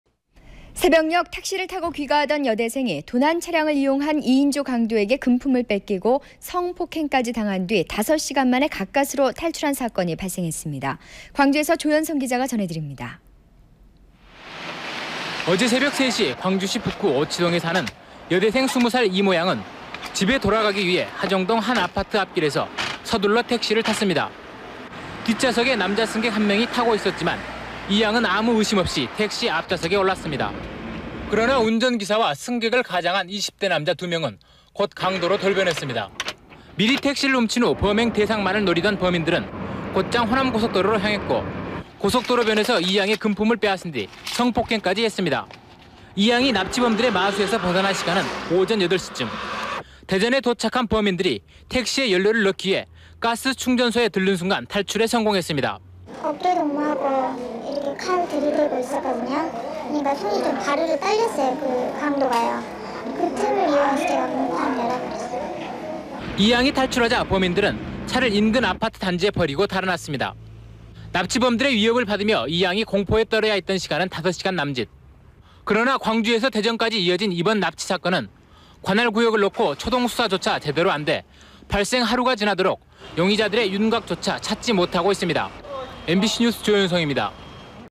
[오!마이] 새벽녁 택시 귀가 여대생, 2인조 강도에게 납치, 성폭행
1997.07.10 새벽녁 택시 귀가 여대생, 택시 훔친 2인조 강도에게 납치
▶대한민국 역대급 캐씁쓸한 사건들 ‘한숨밖에안나와 NEWS‘
▶대한민국 역대급 훈훈한 사건들 ‘감동눈물쓰나미 NEWS'